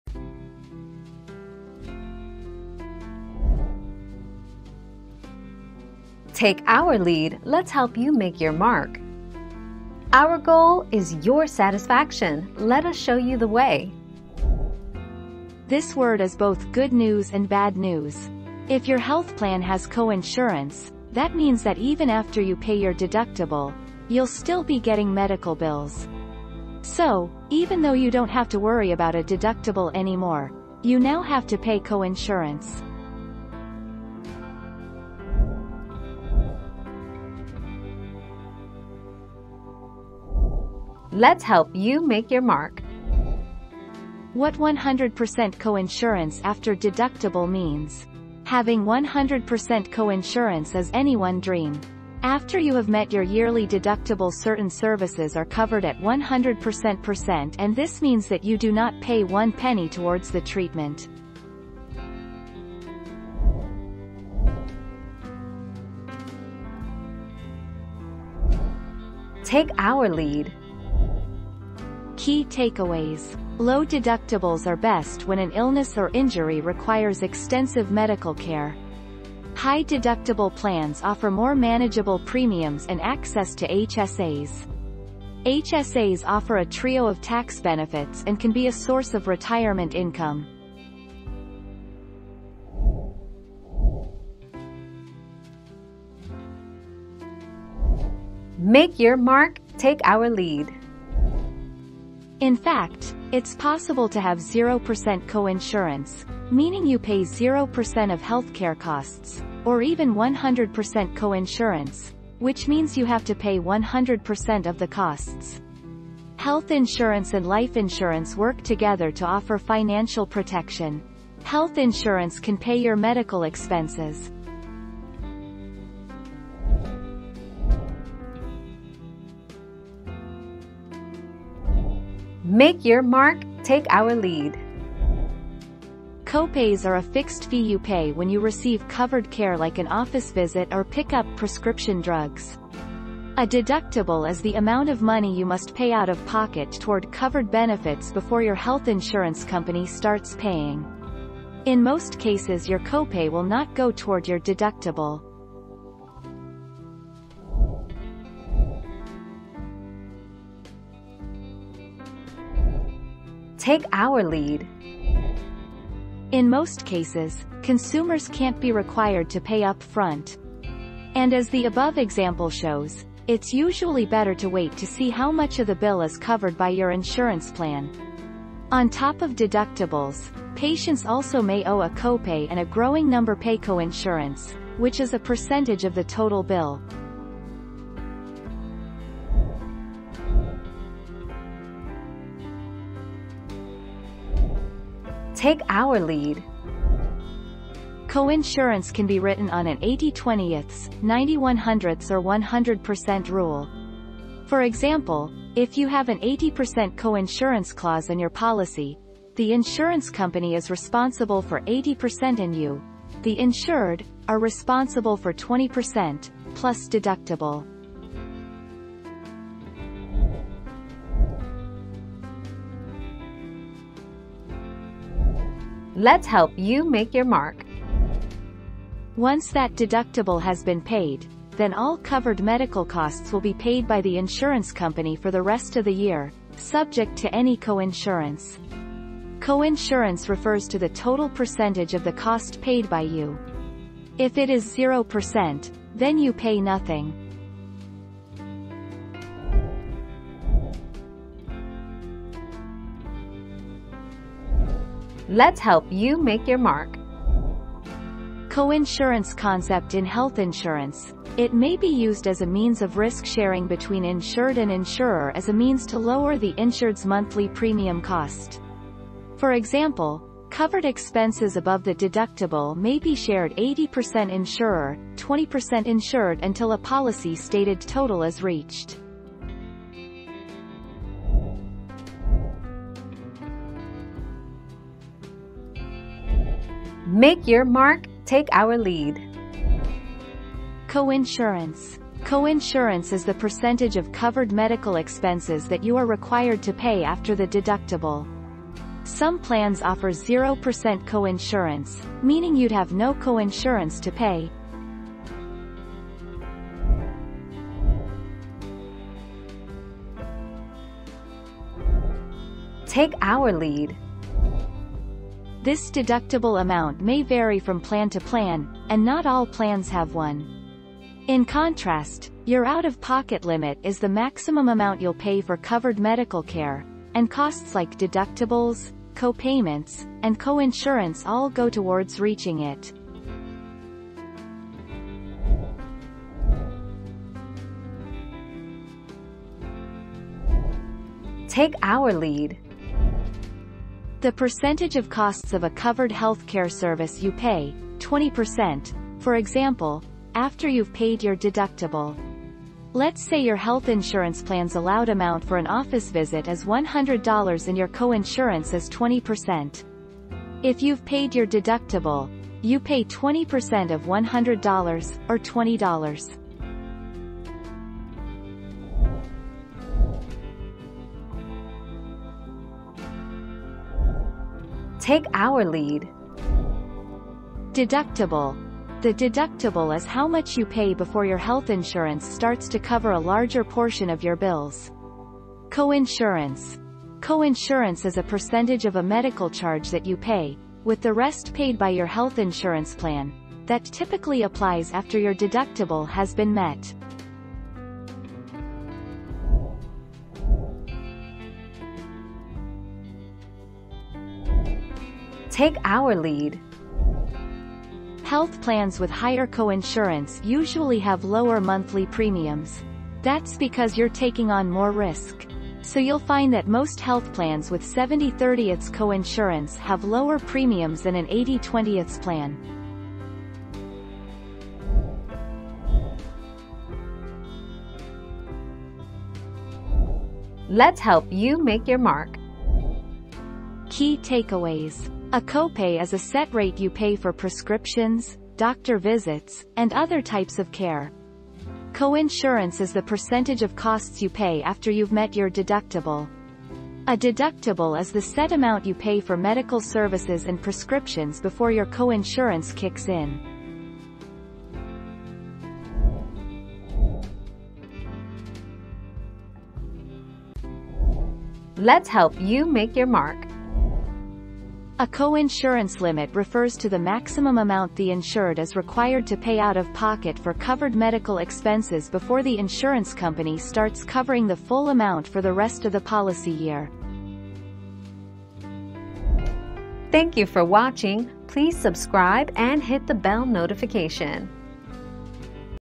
Is coinsurance good or bad?
00:00 - Is coinsurance good or bad?
00:37 - What does this mean 100% coinsurance after deductible?
01:05 - Is it better to have a high deductible or low?
01:34 - What is 100% coinsurance mean?
02:08 - Is it better to have a copay or deductible?
02:38 - Do you pay coinsurance upfront?
03:10 - What does 80% coinsurance mean?
03:41 - Is it good to have 0% coinsurance?
04:10 - What is the point of coinsurance?
04:41 - What does it mean when it says 0 coinsurance?
05:07 - Does coinsurance go towards out of pocket maximum?
05:35 - What is 40% coinsurance deductible?
06:08 - Is coinsurance better than deductible?
06:39 - Is it better to have higher or lower coinsurance?
07:04 - Is it better to have coinsurance or copay?
07:35 - What is coinsurance limit?

Laura S. Harris (2021, April 2.) Is coinsurance good or bad?
    AskAbout.video/articles/Is-coinsurance-good-or-bad-208091